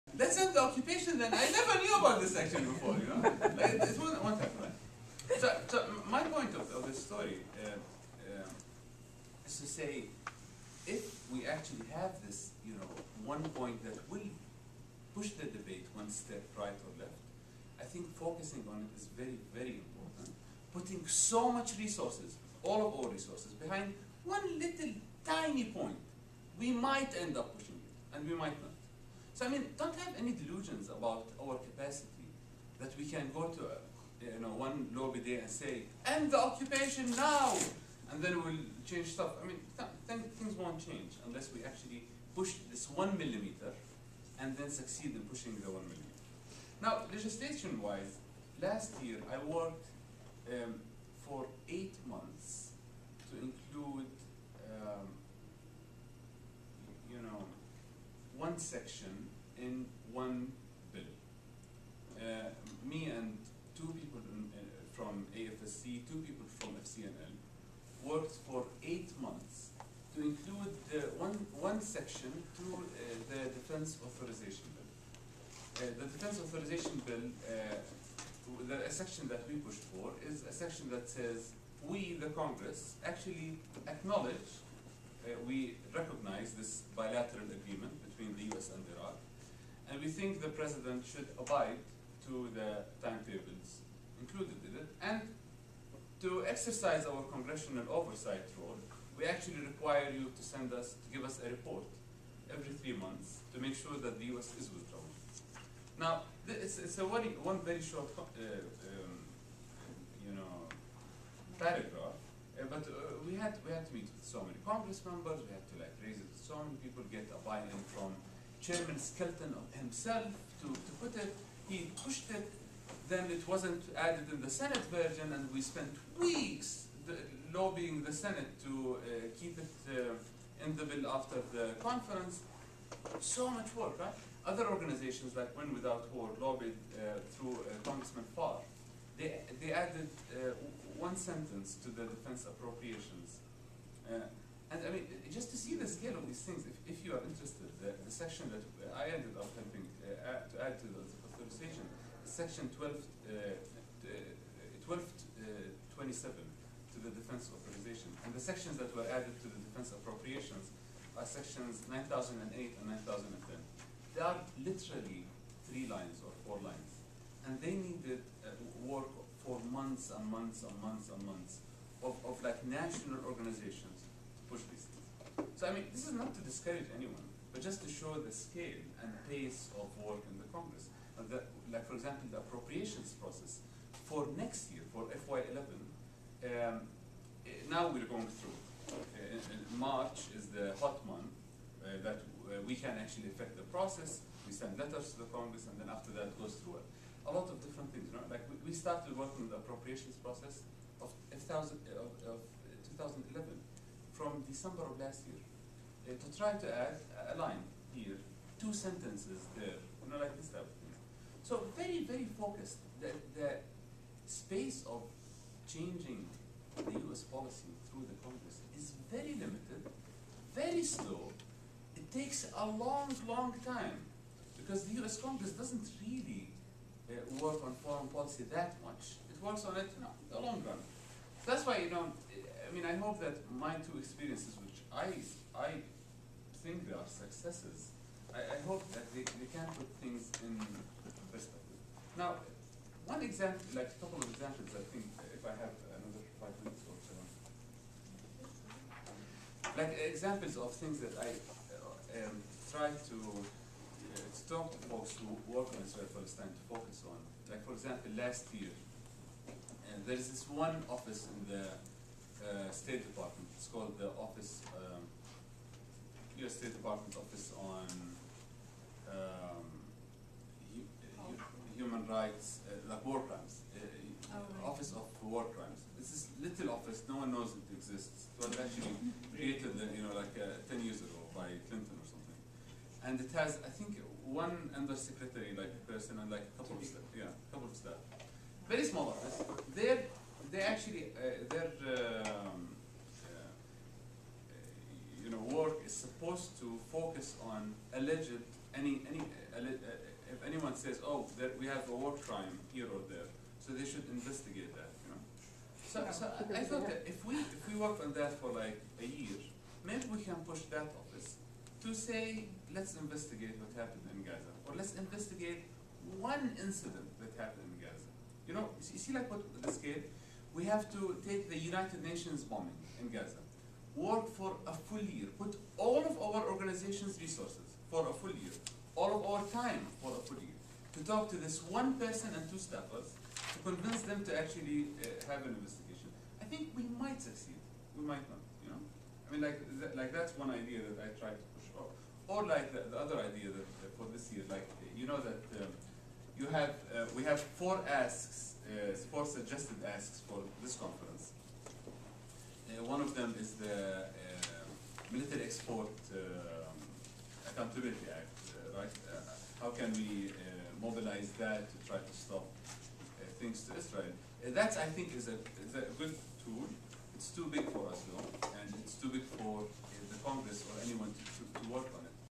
Congressional Engagement Workshop, Part 5
"Congressional Engagement 101:Building Skills and Expanding the Congressional District Coordinator Network" with Josh Ruebner. Workshop 1A at IFPB's Grassroots Advocacy Training & Lobby Day on March 7, 2010.